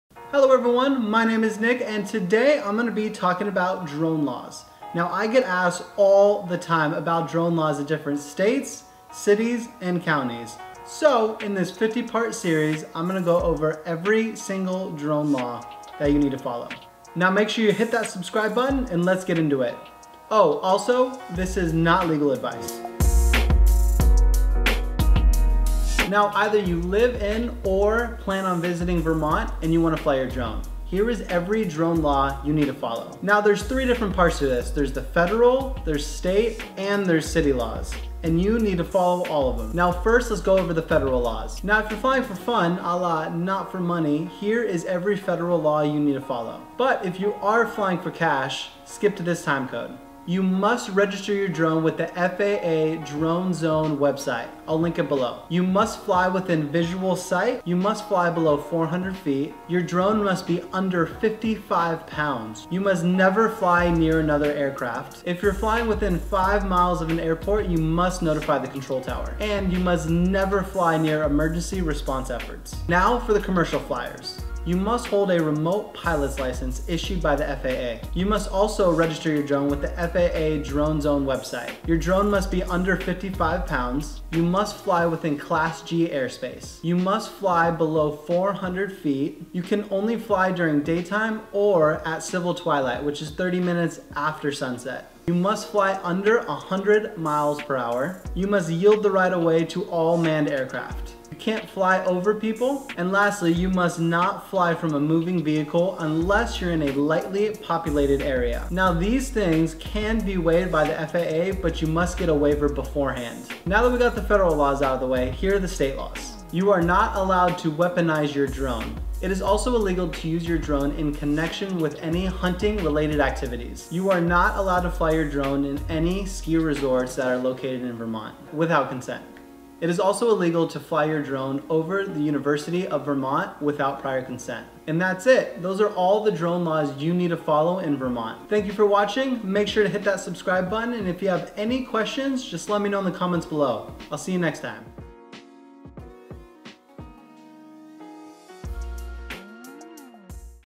Where Can I Fly in Vermont? - Every Drone Law 2019 - Burlington, Rutland (Episode 45)
Not sure where you can fly your drone in Vermont ? In this video we will discuss  every federal, state, and city drone laws as of 2019. This will include Burlington, Rutland, and every city in Vermont .